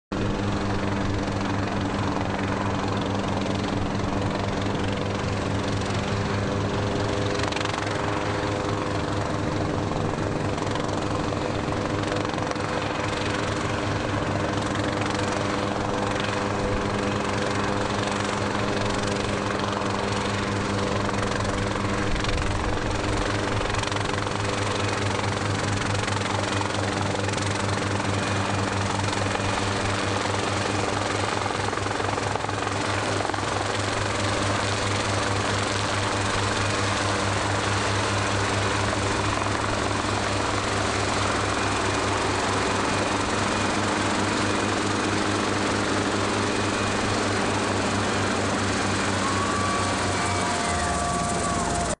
Pawel Achtel- Ng Wholly Owned
A helicopter lands on a ship's helipad.